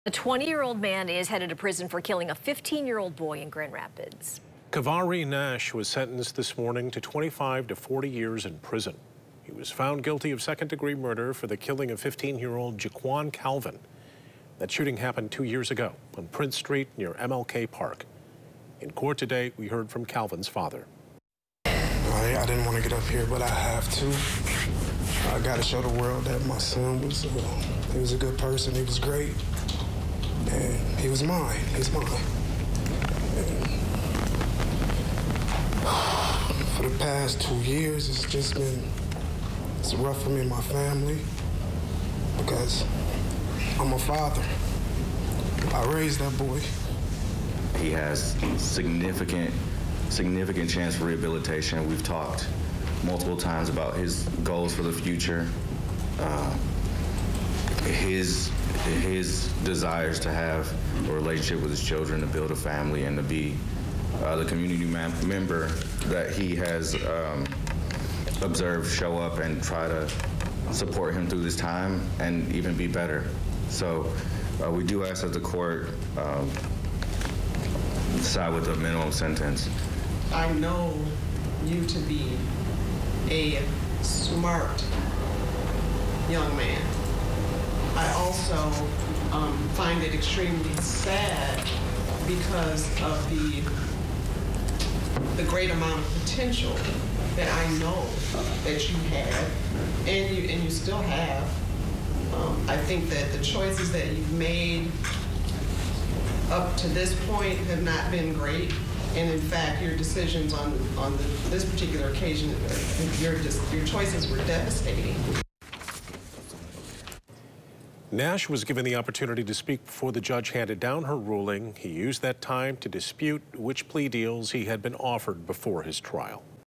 Man sentenced for 2021 murder of Grand Rapids 15-year-old
Khavaree Nash was sentenced Wednesday to 25 to 40 years for second-degree murder in the death of Ju’Qwon Calvin. (April 19, 2023)